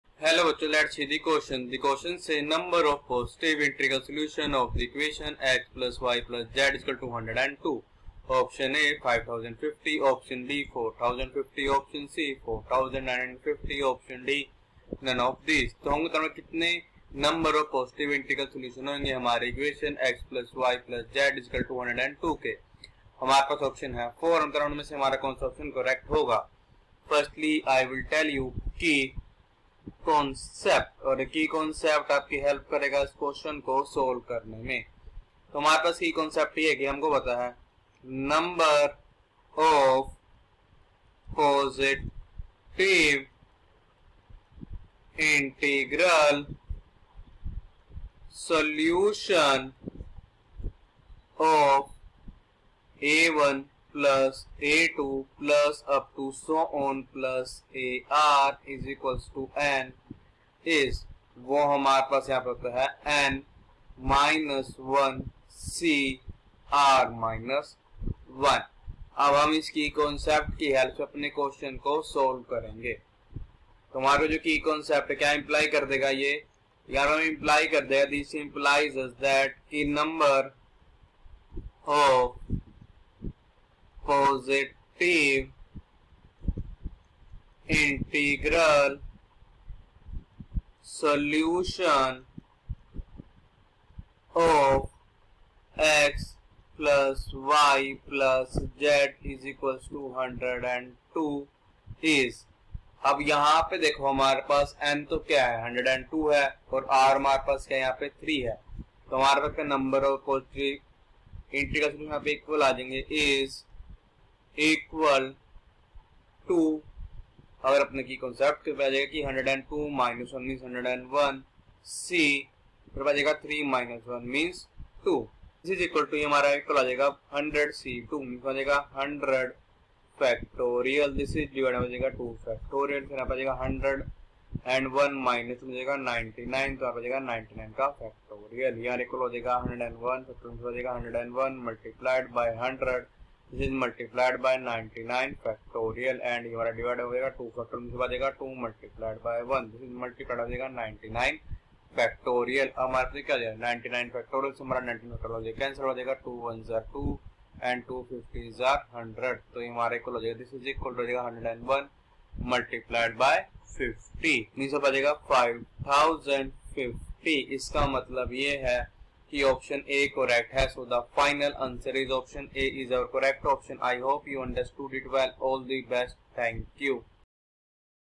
Number of Positive Integral solution of the equation \( x+y+z=102 \). (a) 5050 (b) 4050 (c) 4950...
Number of Positive Integral solution of the equation \( x+y+z=102 \).
(a) 5050
(b) 4050